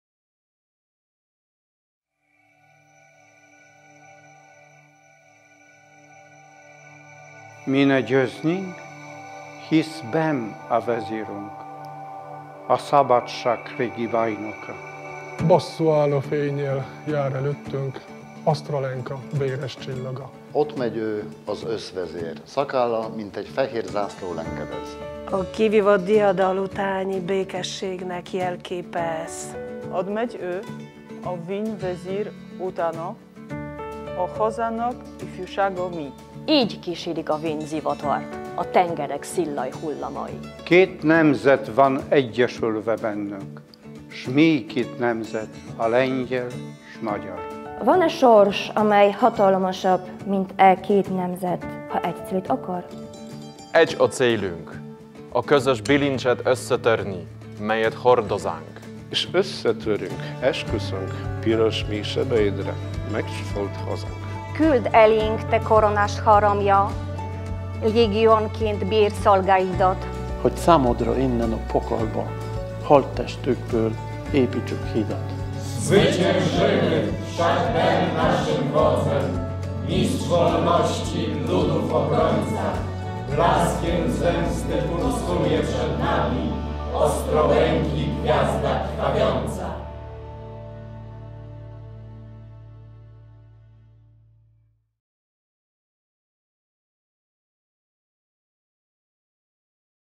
MÁRCIUS 15. Lengyel diplomaták: Petőfi Sándor "Az erdélyi hadsereg"
A magyarországi lengyel diplomaták üdvözletüket küldik a magyaroknak március 15-e nemzeti ünnepe és a közeledő Lengyel-Magyar Barátság Napja (március 23-24, Veszprém) alkalmából.